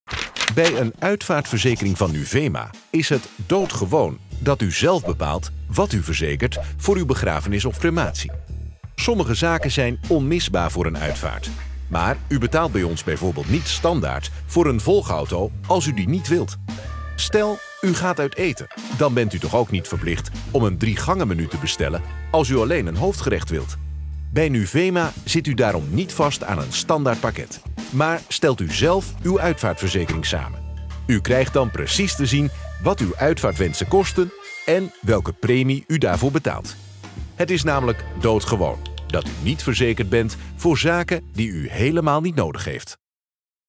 Uitvaartverzekering met eigen wensen
Alleen bij Nuvema is het doodgewoon dat u zelf kiest welke uitvaartwensen u wilt verzekeren. U bent toch ook niet verplicht om een driegangenmenu te bestellen als u alleen het hoofdgerecht wilt?

Met de cartoon Doodgewoon proberen we de dood bespreekbaar te maken. Wij vinden het namelijk belangrijk dat mensen nadenken over hun uitvaart.